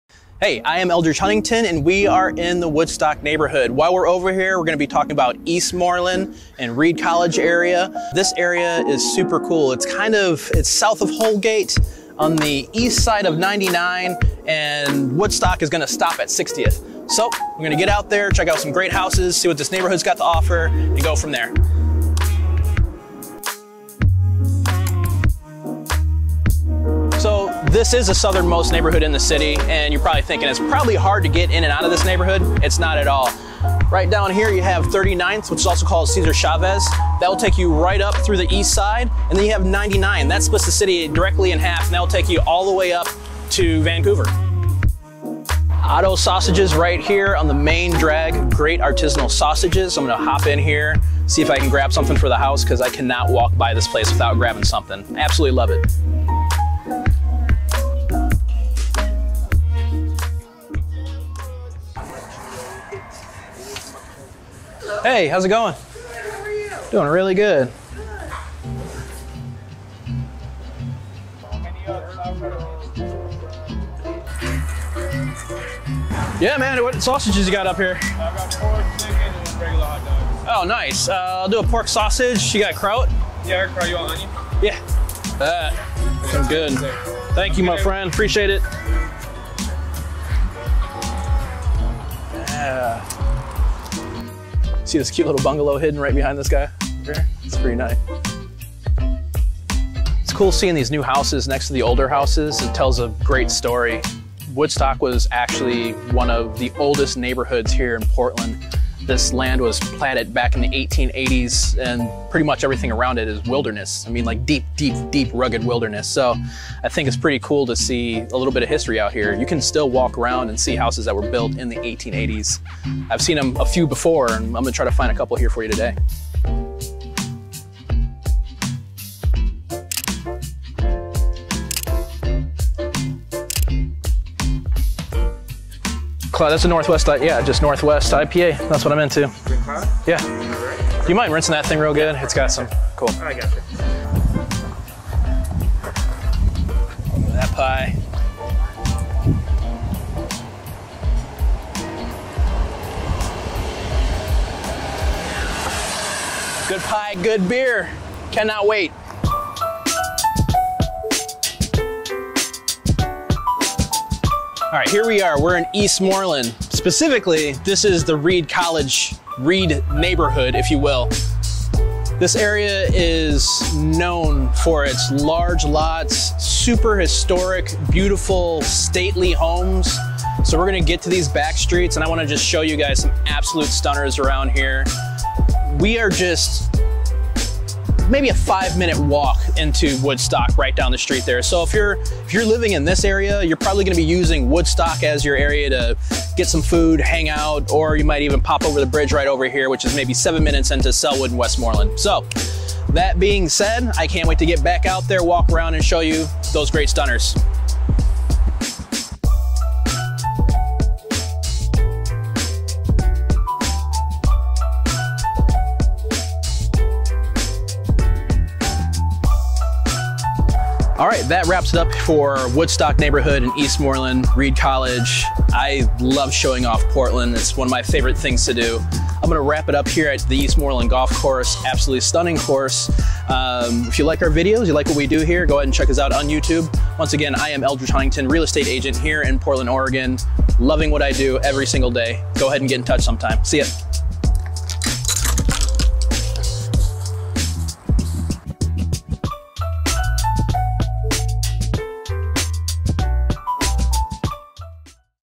EXPLORE Woodstock (SE PDX) - HOUSE HUNTER PDX
Welcome back to another episode of the EXPLORE PDX series! This time we're exploring one of Portland's oldest neighborhoods: Woodstock!

Fun Fact, there are still homes standing in Woodstock that were built in the 1880s. The neighborhood is RICH in history. Come and explore the area with me.

If you're in the market for a Portland home, get in touch!